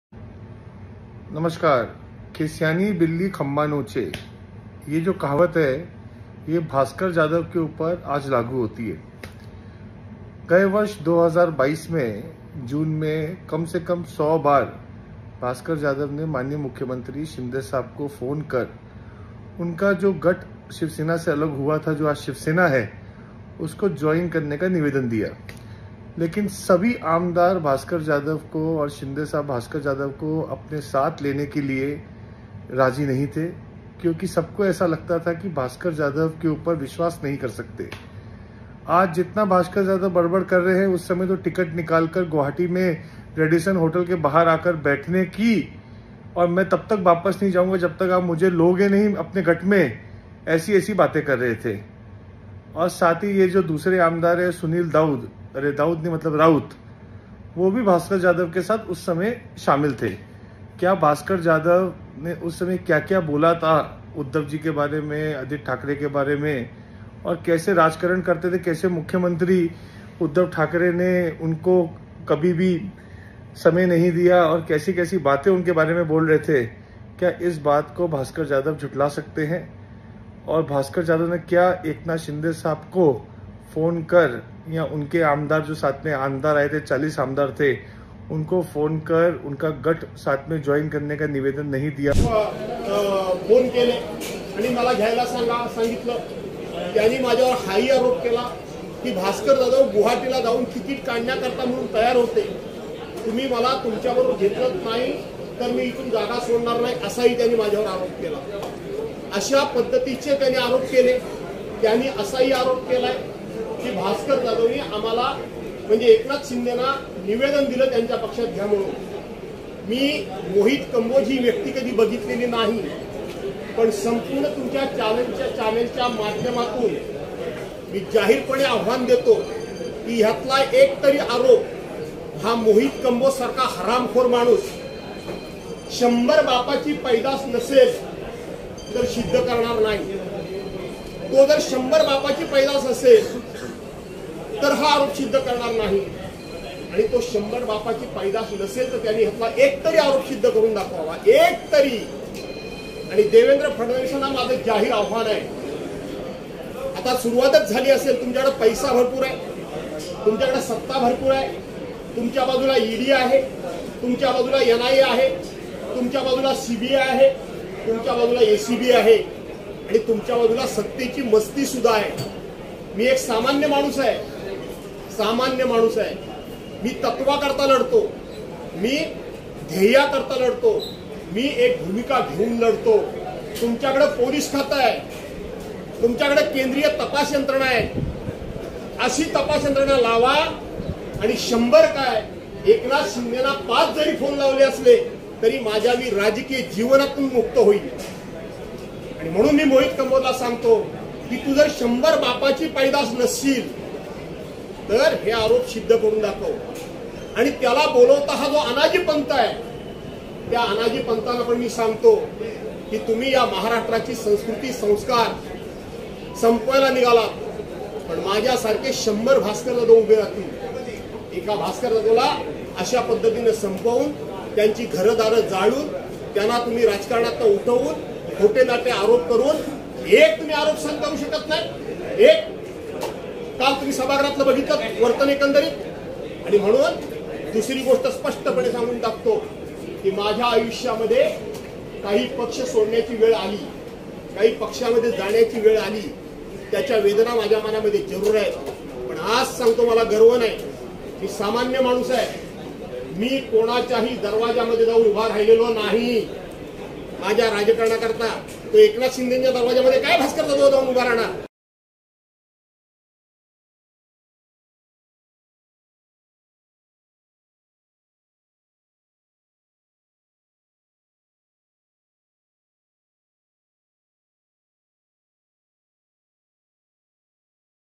Mohit Kaboj : कंबोज यांचा दावा, जाधवांचे ओपन चॅलेंज... : LetsUpp Marathi
भाजपचे मोहित कंबोज यांनी ठाकरे गटाचे आमदार भास्कर जाधव यांच्यावर अनेक गंभीर आरोप केले. त्यांनी केलेल्या आरोपांवरून जाधव चांगलेच भडकल्याचे दिसून आले.